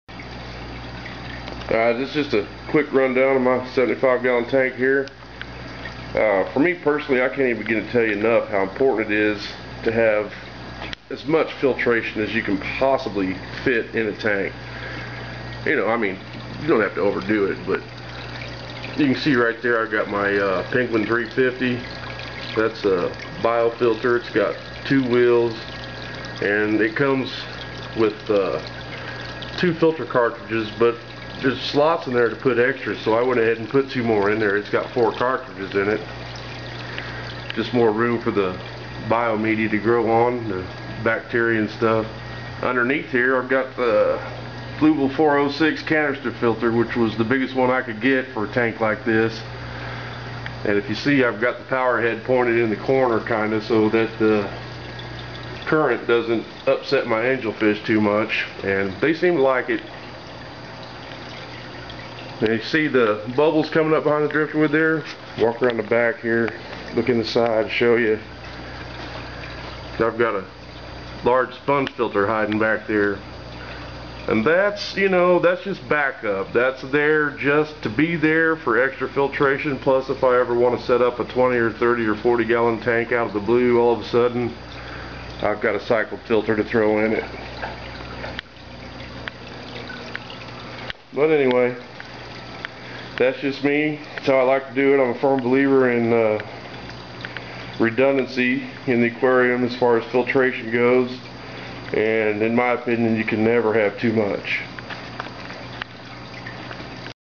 Massive filtration, redundancy is essential to happy fish keeping!
Just a quick example of the redundancy in filtration in my 75 gallon tank. I do this wih all my tanks and I think everyone should!